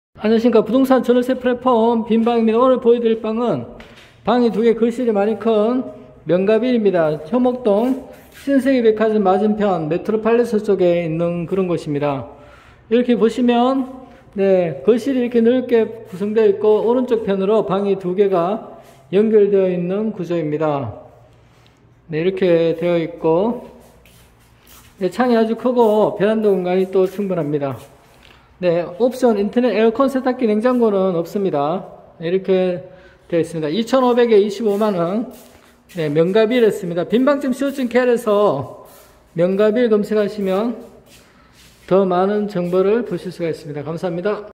효목동 명가빌 쓰리룸 2000/40
에서 "명가빌"로 검색해 보세요.

진로 이스트 바로 옆.

정보
위치 대구광역시 동구 효목동 349-1번지
도로명 대구광역시 동구 효목로13길 11
쓰리룸 면적 50㎡
방향 남동향
입주가능일 즉시
방수/욕실수 2/1
난방 도시가스
기타용도 제2종근린생활시설(소매점,사무소)
층수 지상4층/지하0층
승강기수 0기
구조 철근콘크리트구조
세대/가구 0세대/ 3가구
대지면적 196.1㎡
연면적 421.86㎡ (용적율 215.12%)
건축면적 116.13㎡ (건폐율 59.22%)
주차 옥외 0대, 옥내 0대
사용승인일 20061010
건축물대장 층별개요
주용도 층 면적
다가구주택 지상 4층 87.69㎡
다가구주택 지상 3층 101.91㎡
사무소 지상 2층 116.13㎡
소매점 지상 1층 116.13㎡

대구광역시 북구 복현로 32-1 캠퍼스부동산중개사무소 | 대표자: 김성택
사업자등록번호: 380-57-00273 | 부동산등록번호: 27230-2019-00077